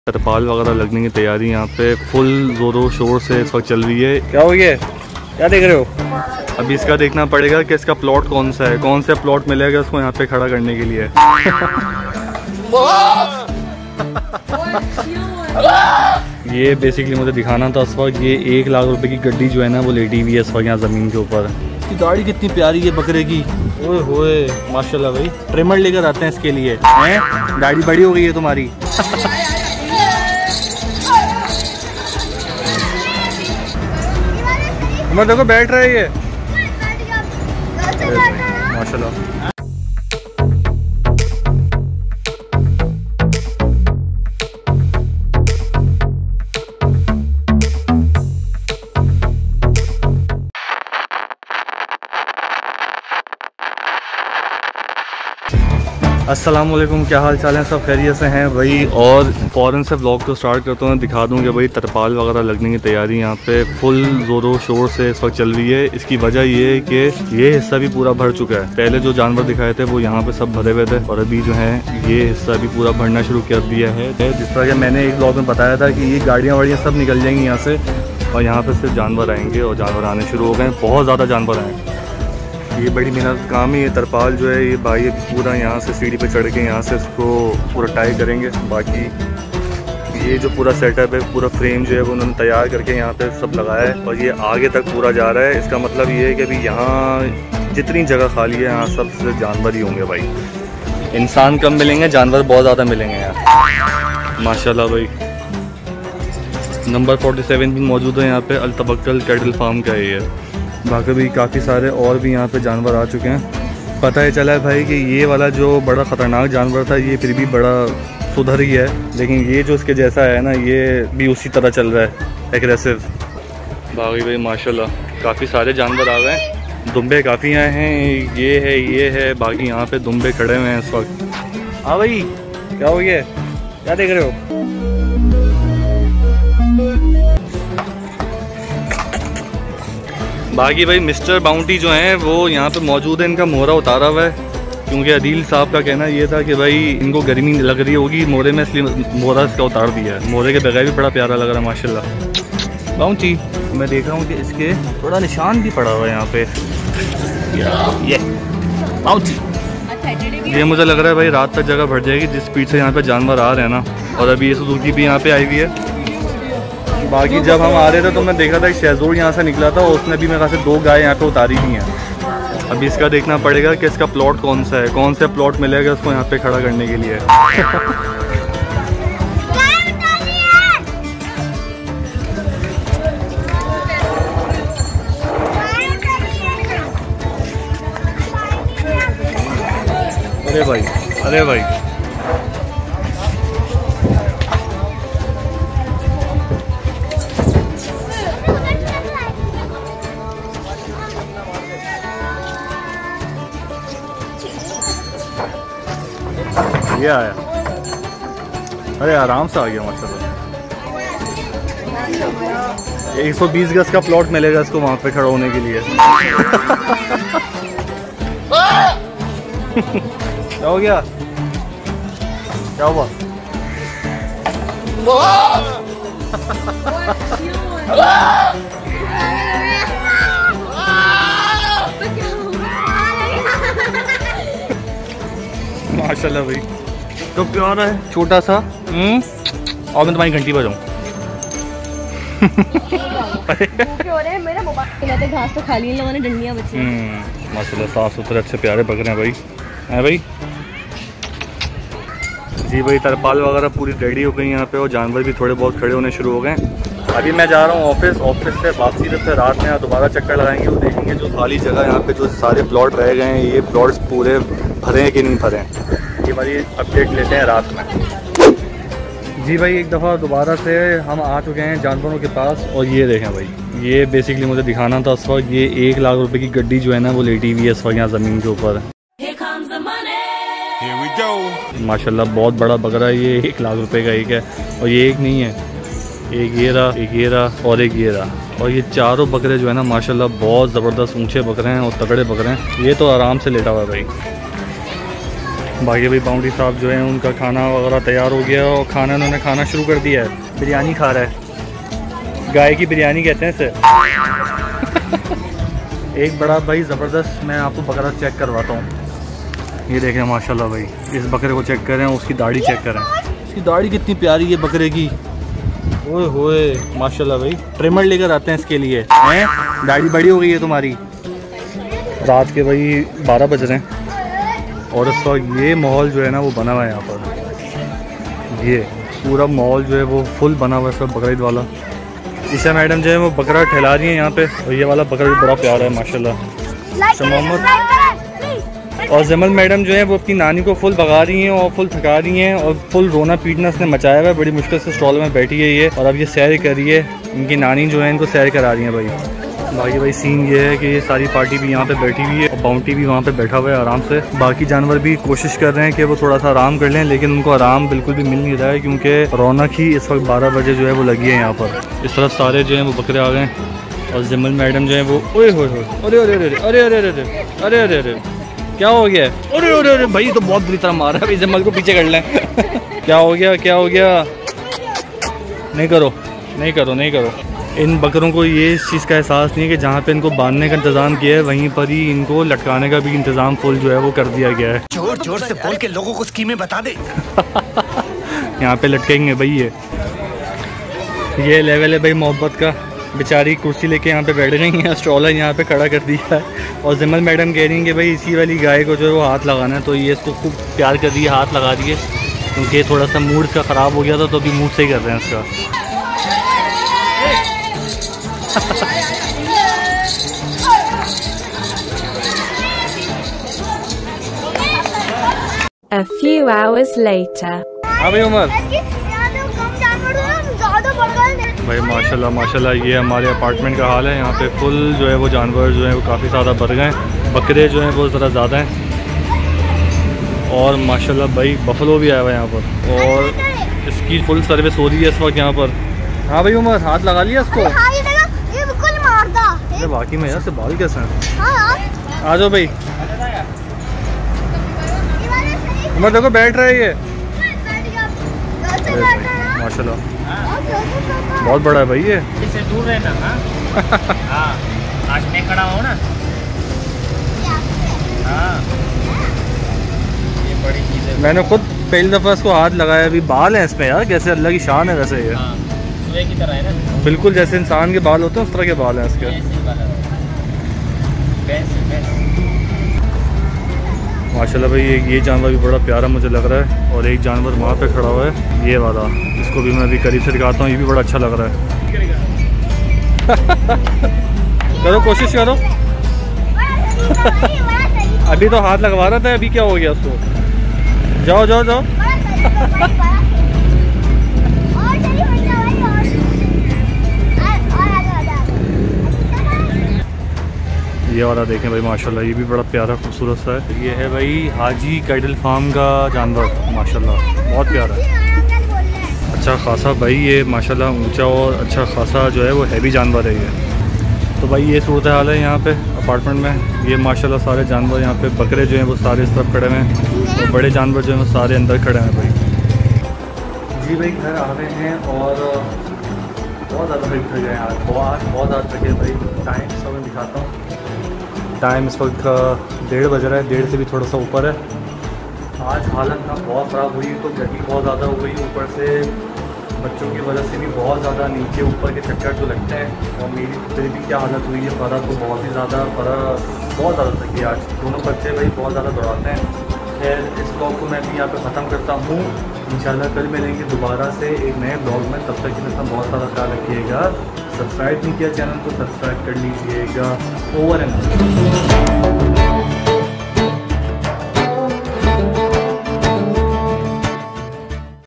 Qurbani Kay Itnay Ziada Janwar | Aisa Bakra Nahi Dekha
Hi this is fahad butt. Itnay janwar agai hain kay ab kharay kahan keraingay?